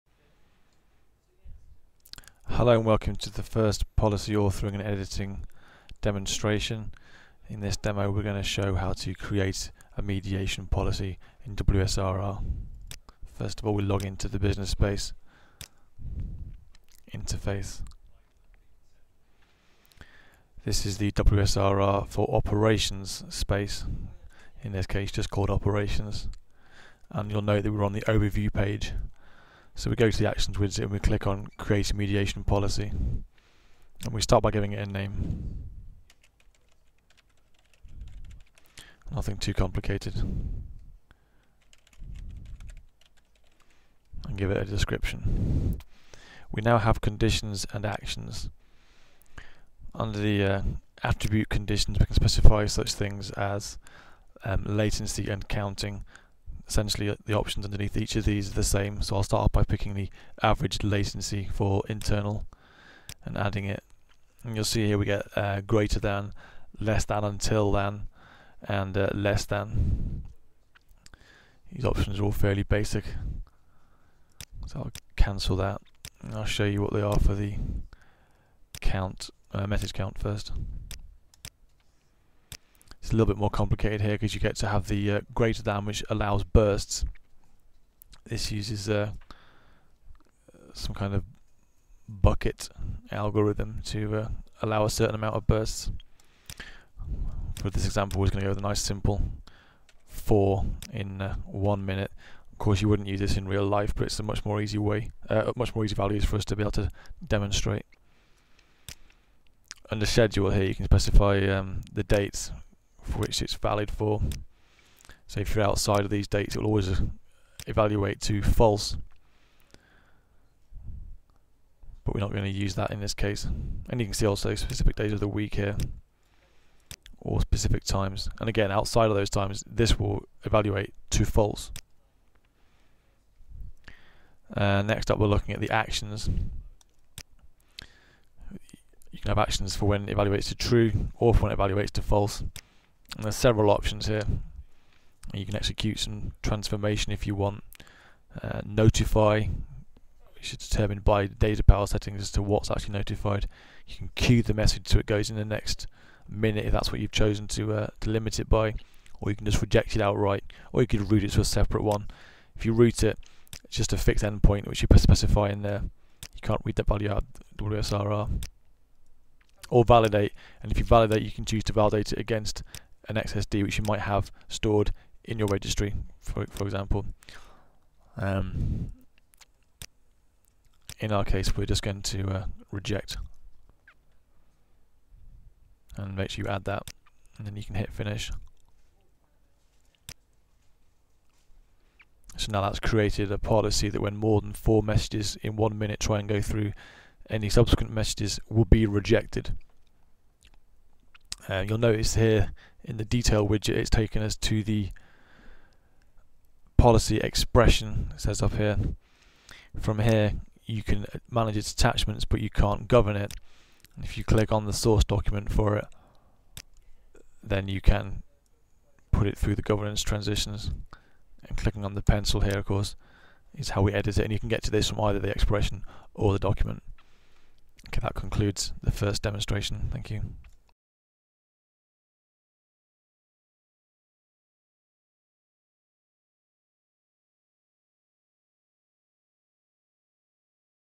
Mediation Policy Authoring and Editing in WebSphere Service Registry and Repository V8.0 (1/4)
This demonstration shows the use of the WebSphere Service Registry and Repository (WSRR) V8.0 Business Space User Interface to author and edit Mediation Policies.

A mediation policy is used to specify acceptable performance criteria for a service; the policy performs actions depending on whether the test of a criteria passes or fails. You can also specify a policy which does not test conditions, and always completes the specified action.

In this demonstration you will see:
    How to author a Service Level Mediation policy in WebSphere Service Registry and Repository V8.0, including
        the various attribute conditions
        the scheduling conditions
        and the actions which will occur when the conditions evaluate to true or false
    The differences between the Policy Document and the Policy Expression.

The postings on this channel are our own and don't necessarily represent IBM's positions, strategies or opinions.